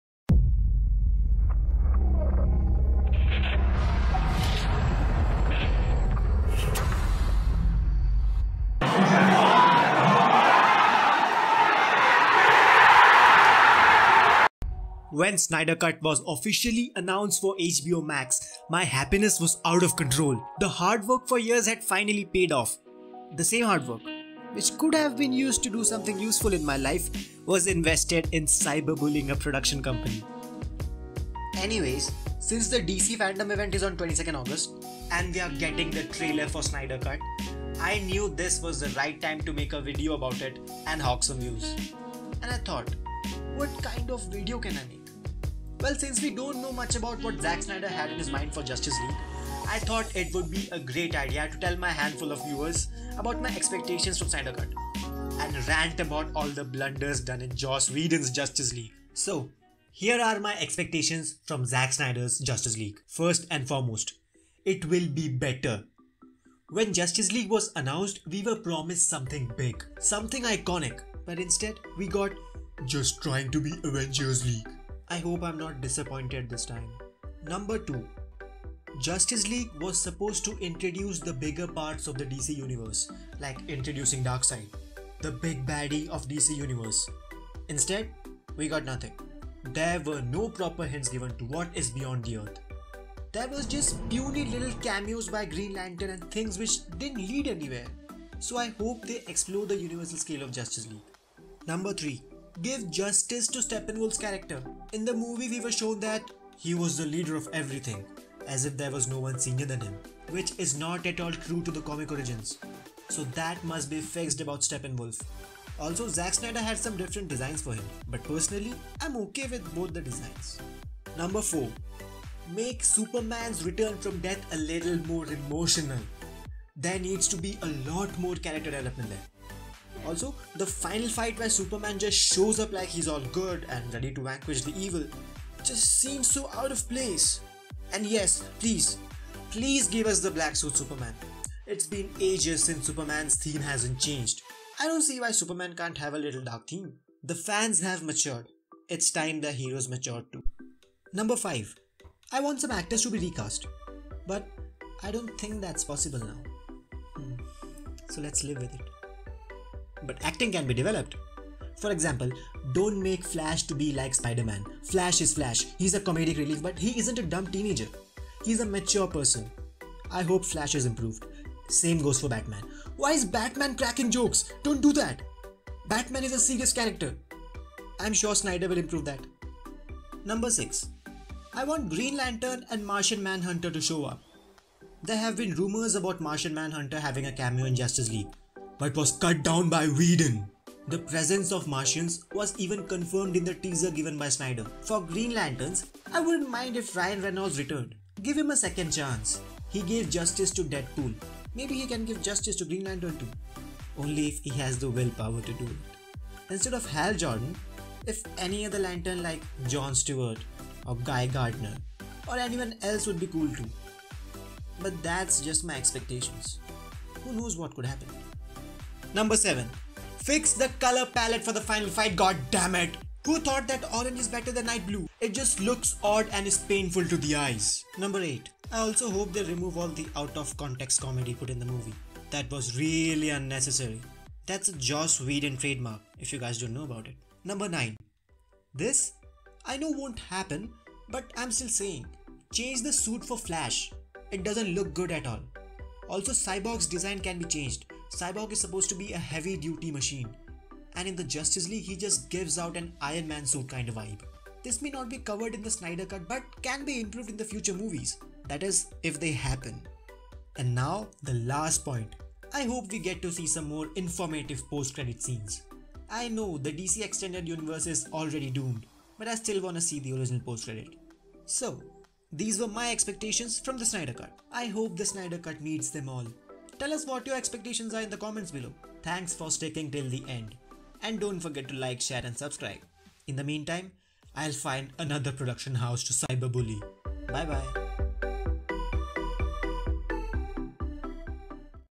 Expectations From The Snyder Cut (Joss Whedon Rant)
Hello fellow weebsters (ﾉ◕ヮ◕)ﾉ*:･ﾟ✧
► Joss Whedon screwed up justice league real bad.. and after that.. snyder cut being announced is a dream come true... so, these are my expectations from the Snyder cut.. hope that the show lives upto it!!!
Here are the timestamps to every point!!
Start            0:00
Intro            0:14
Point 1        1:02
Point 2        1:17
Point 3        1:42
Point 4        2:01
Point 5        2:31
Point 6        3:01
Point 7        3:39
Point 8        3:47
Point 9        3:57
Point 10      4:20
Ending         4:30
Outro Neko 4:47

You could literally interact with us on our subreddit and have a chill time!